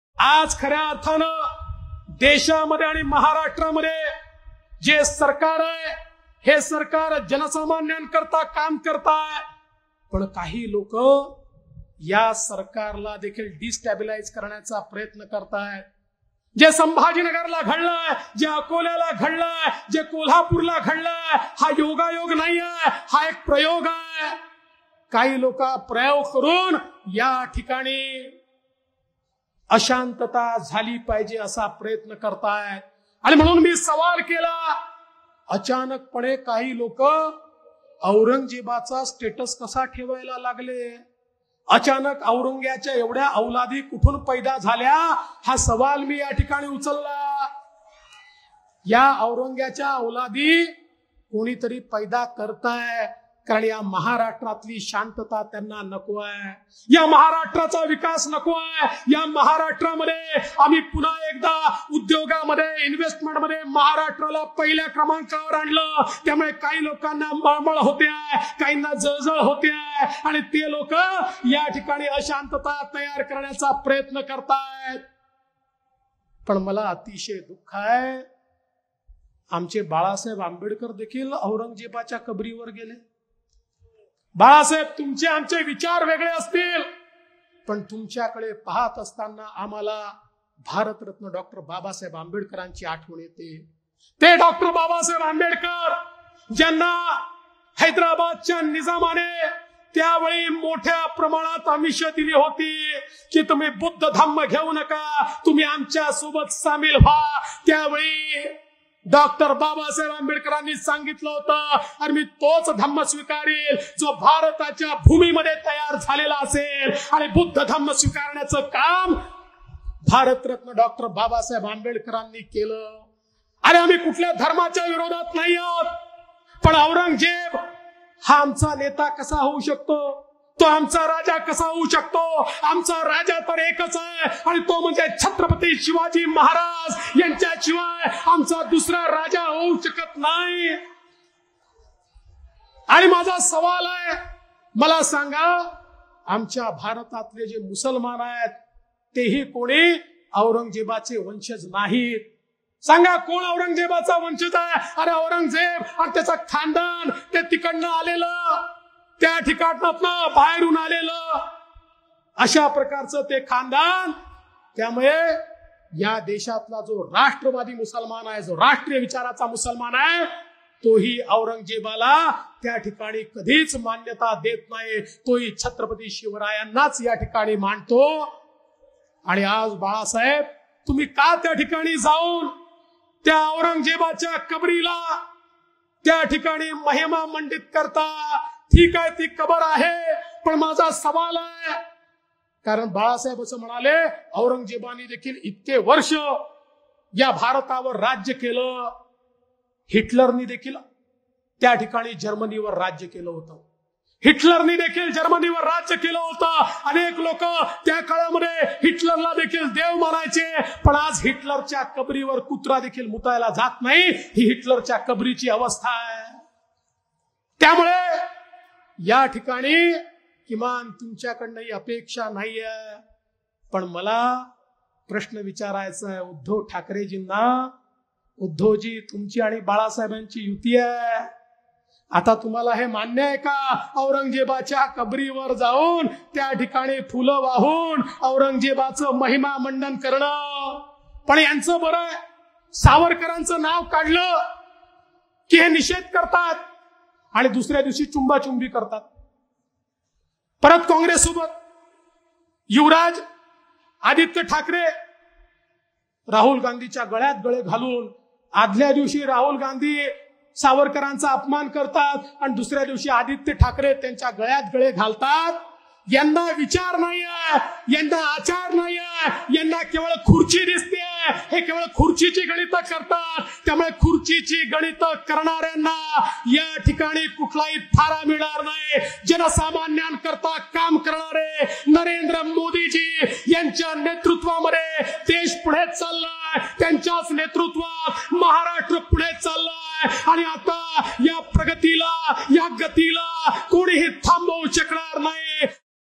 काही जिल्ह्यांमध्ये अशांतता माजत आहे हा योगायोग नाही तर प्रयोग! | अकोला | DCM Devendra Fadnavis
(Modi@9 Grand Public Meeting, Akola | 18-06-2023)